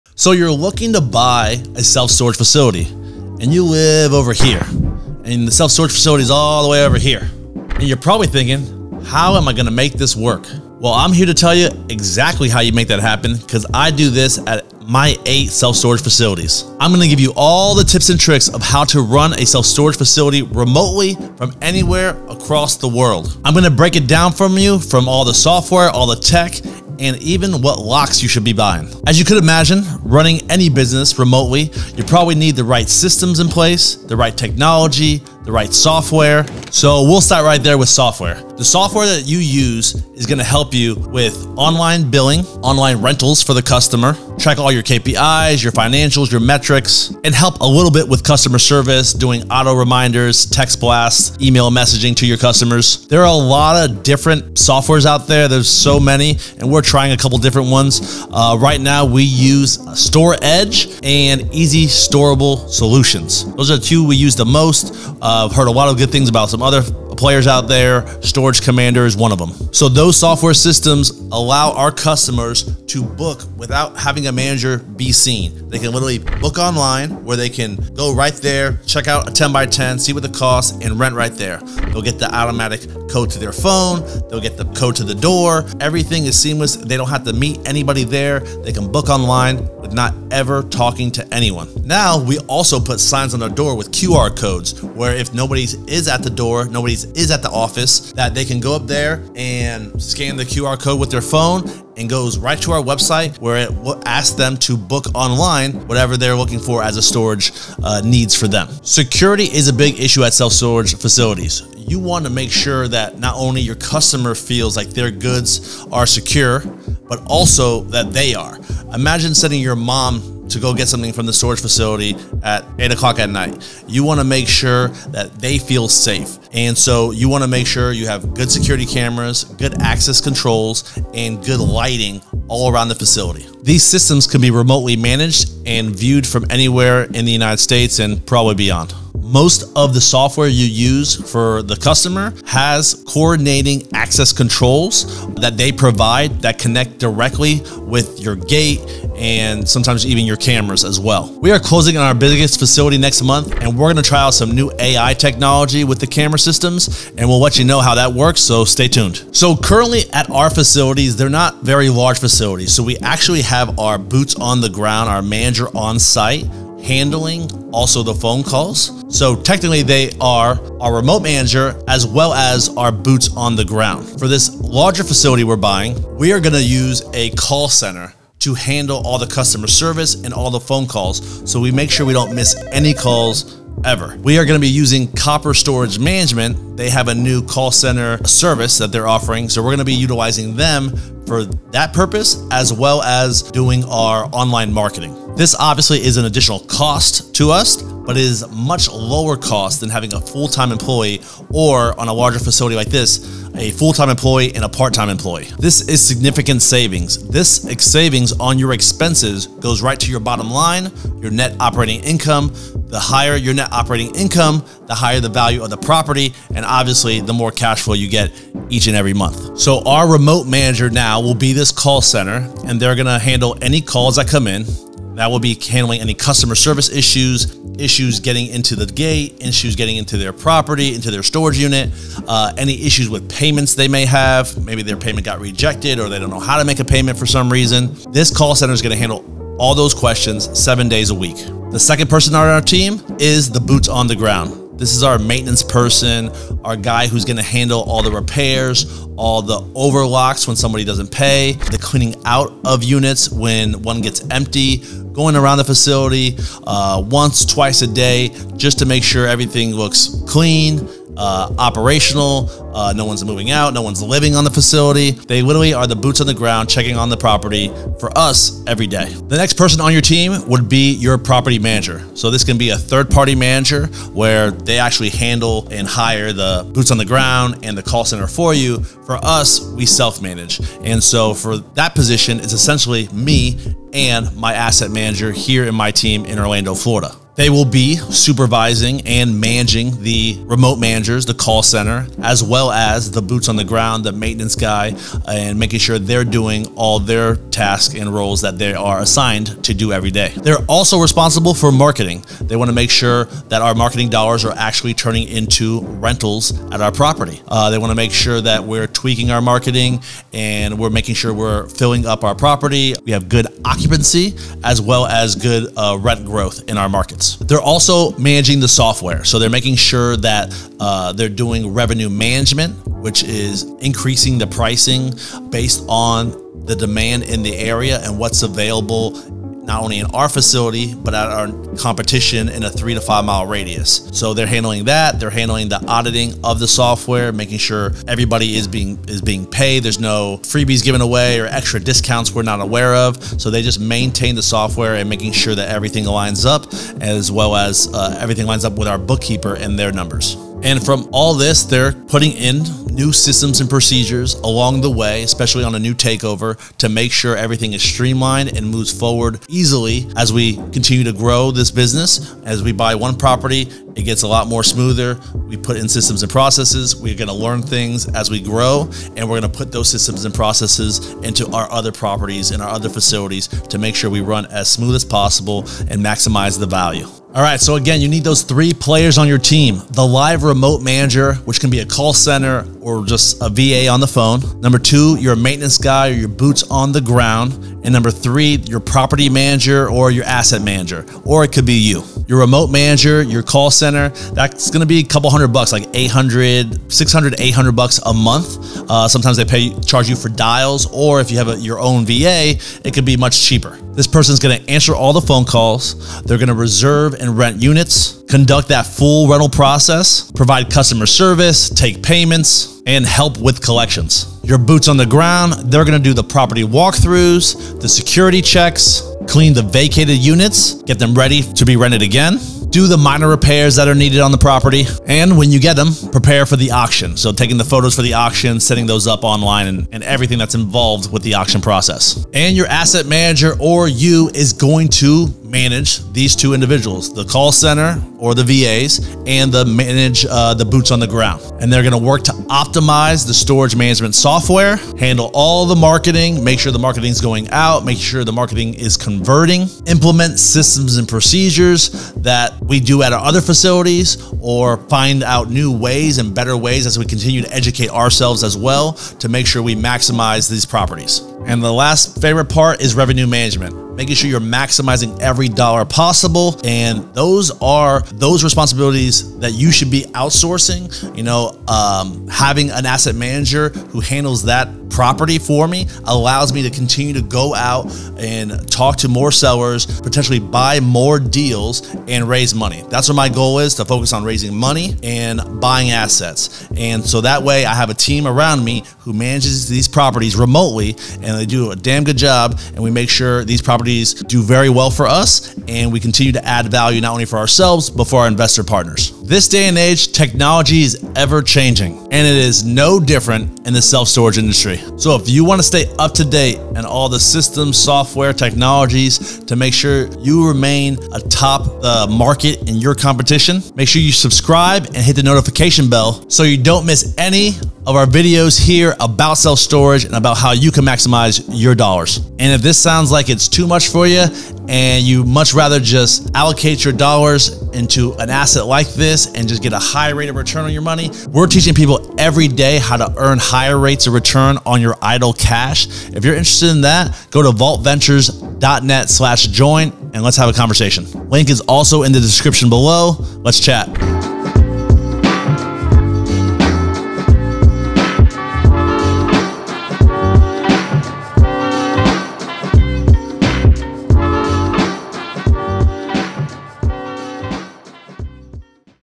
Running a Self Storage Facility Remotely | Everything You Need To Know
In this video, we'll cover everything you need to know about running a self-storage facility remotely. Whether you're an entrepreneur looking to expand your business or an investor interested in owning a storage facility, you can manage it from anywhere with the right tools and team.

We'll discuss the latest software and technology that can help you streamline your operations, automate payments and rentals, and keep track of inventory and customer data. We'll also go over the key people you'll need to hire, such as a reliable manager, security personnel, and maintenance staff, to ensure your facility runs smoothly and efficiently.

So, if you're ready to learn how to run a self-storage facility from anywhere, don't miss this video!

Timecodes
0:00 Video Intro
0:38 Software
2:45 Who You Need to Hire
7:43 Revenue Management
8:22 Video Recap
8:40 Get High Returns on Your Capital

Self-Storage Software

Locks

Call Center Service